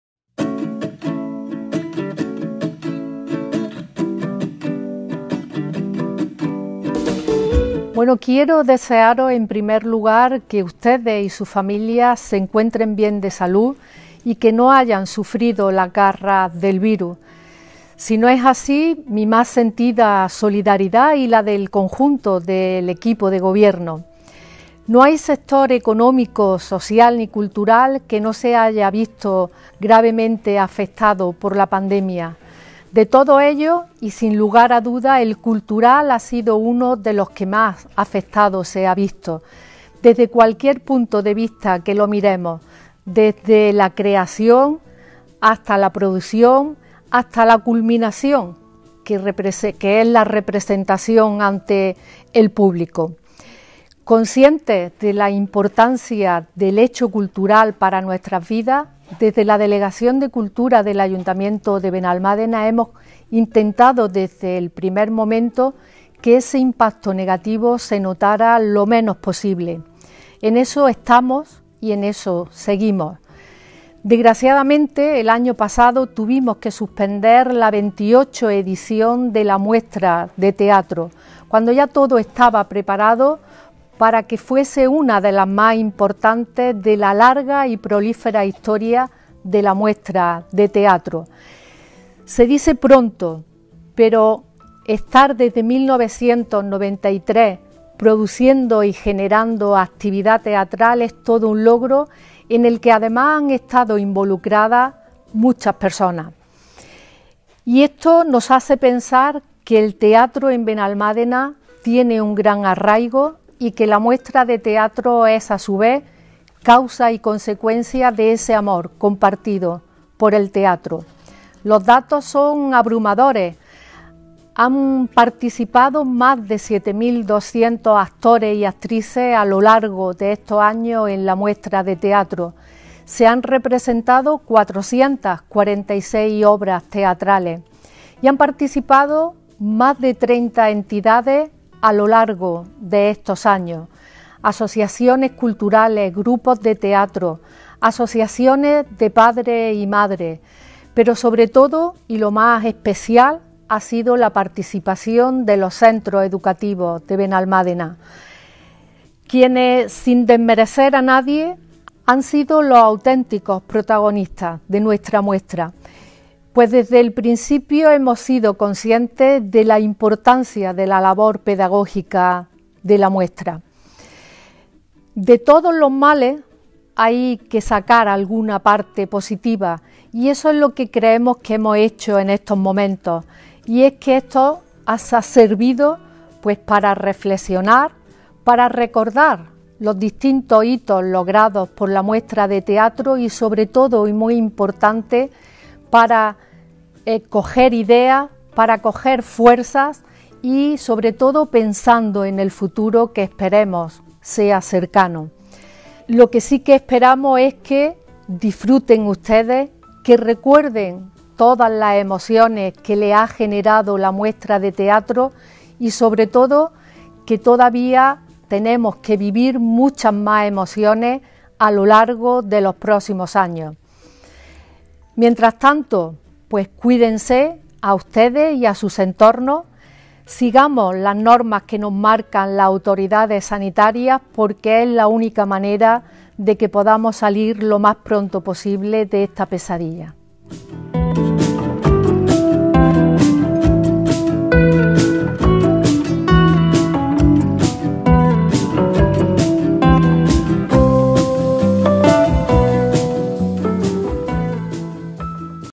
MUESTRA DE TEATRO DE BENALMÁDENA “Emocionándote desde 1993”. Introducción por Concejala Elena Galán
MUESTRA DE TEATRO DE BENALMÁDENA “EMOCIONÁNDOTE DESDE 1993”

En el año 2021, ante la imposibilidad de disfrutar de las representaciones en vivo, recuperamos algunos de los momentos vividos en la Muestra de Teatro de Benalmádena, y damos voz a sus protagonistas.
Introducción por la Concejala de Cultura Elena Galán.